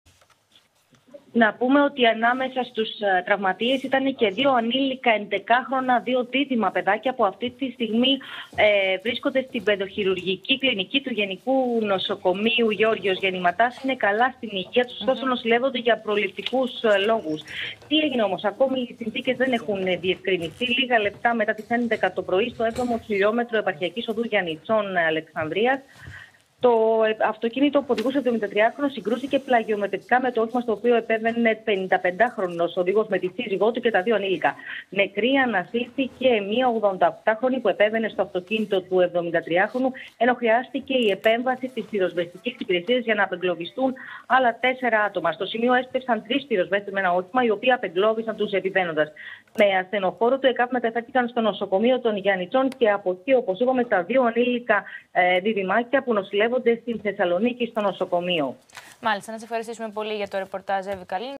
Γιαννιτσά: Τροχαίο με μία νεκρή και έξι τραυματίες
Με αίμα βάφτηκε για μια ακόμα φορά η άσφαλτος, αυτή τη φορά στην περιοχή των Γιαννιτσών Πέλλας. Κάτω από αδιευκρίνιστες συνθήκες, λίγα λεπτά μετά τις 11:00 το πρωί της Κυριακής (27/10) δύο ΙΧ συγκρούστηκαν πλαγιομετρικά στο 7ο χλμ της Επαρχιακής Οδού Γιαννιτσών Αλεξάνδρειας.

Συνέπεια της σύγκρουσης ήταν να χάσει τη ζωή της μια 88χρονη γυναίκα και να τραυματιστούν άλλα έξι άτομα, μεταξύ των οποίων και δύο ανήλικα.

Μάλιστα για τον απεγκλωβισμό της ηλικιωμένης αλλά και τεσσάρων τραυματιών χρειάστηκε επιχείρηση της Πυροσβεστικής Υπηρεσίας.

Οι έξι τραυματίες παρελήφθησαν από ασθενοφόρα του ΕΚΑΒ και μεταφέρθηκαν στο νοσοκομείο Γιαννιτσών.

Προανάκριση για τα ακριβή αίτια του τροχαίου δυστυχήματος διενεργεί το Τμήμα Τροχαίας Πέλλας.